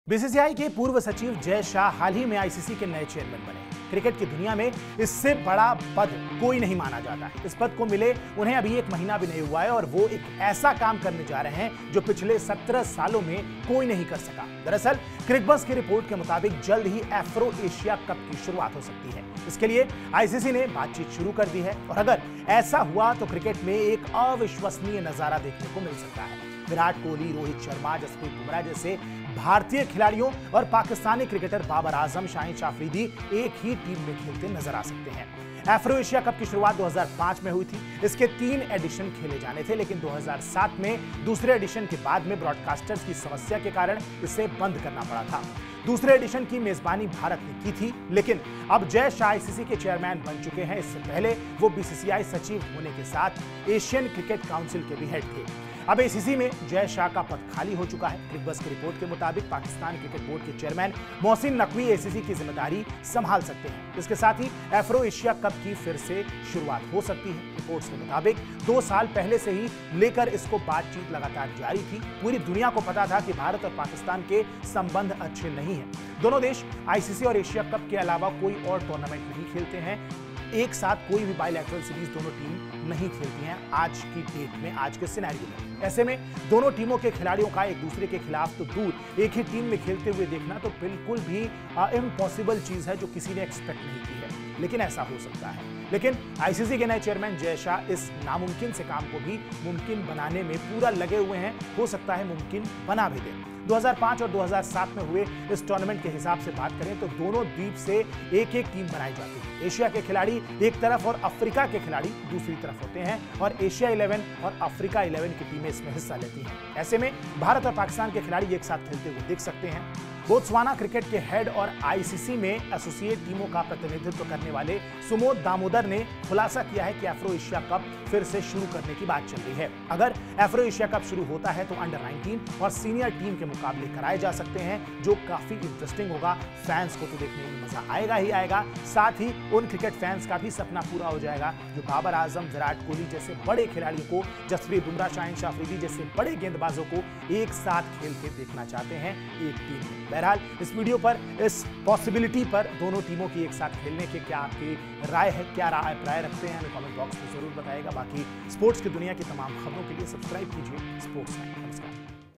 Jay Shah का Master Plan! एक ही टीम में खेलेंगे Virat Kohli और Babar Azam? | Afro Asia Cup| ICC| BCCI
बीसीसीआई के पूर्व सचिव जय शाह 17 सालों के बाद एफ्रो-एशिया कप की शुरुआत कर सकते हैं. अगर ऐसा होता है भारत और पाकिस्तान के खिलाड़ी एक ही टीम में खेलते हुए दिख सकते हैं. यानि विराट कोहली और जसप्रीत के साथ बाबर आजम और शाहीन अफरीदी एक ही टीम में खेलते हुए दिख सकते हैं.